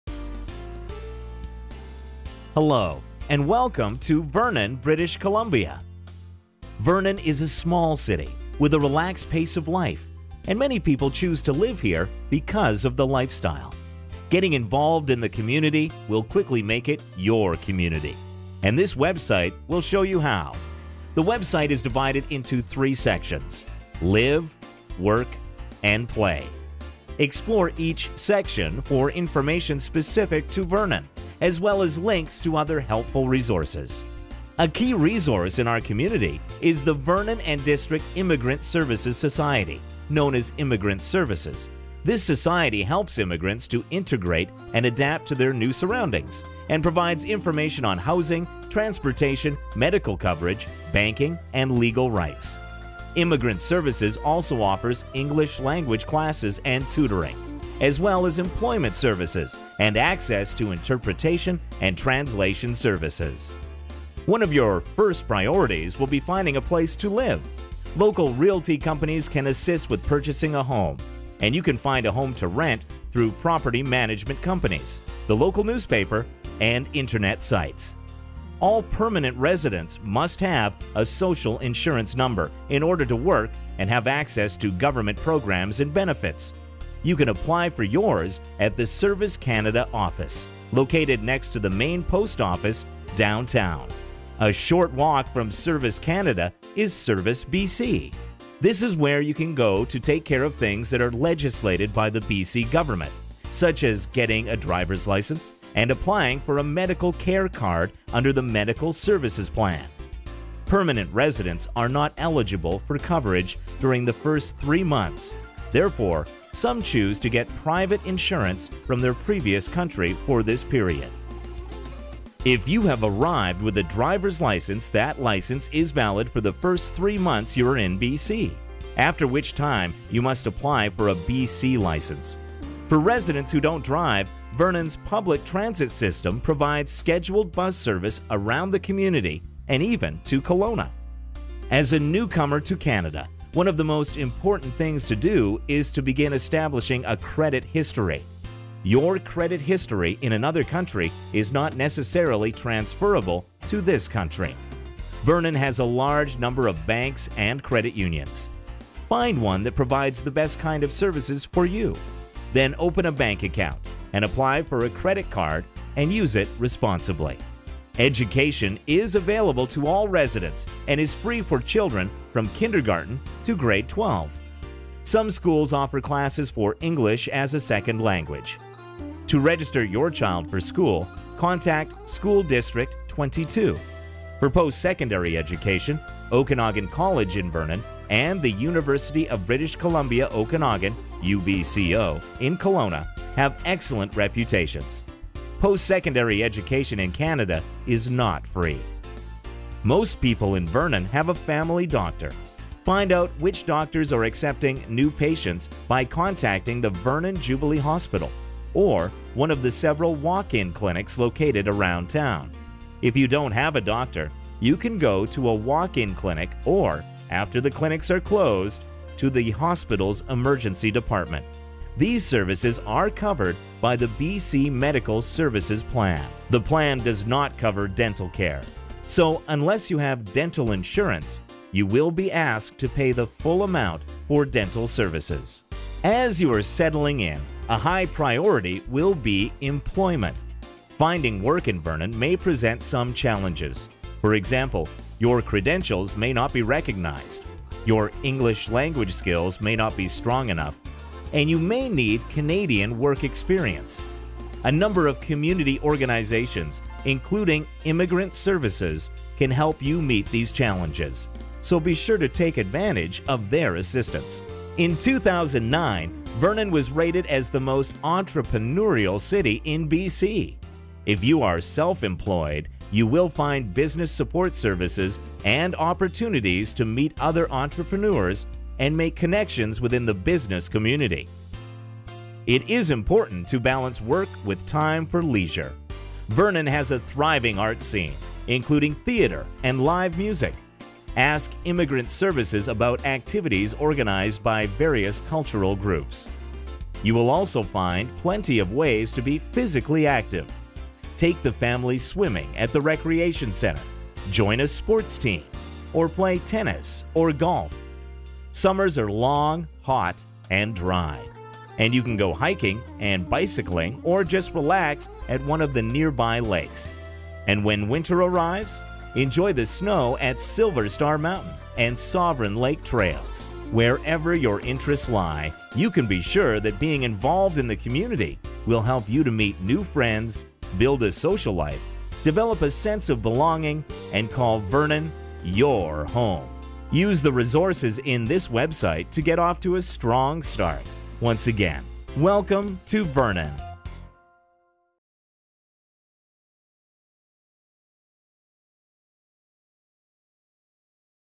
Welcoming Communities Vernon BC | English
Welcome Vernon introduces new immigrants to living, working, and playing in Vernon, British Columbia. It builds on the British Columbia Newcomers' Guide to Resources and Services, Vernon Edition, and includes information on the community, local resources, and the Canadian workplace.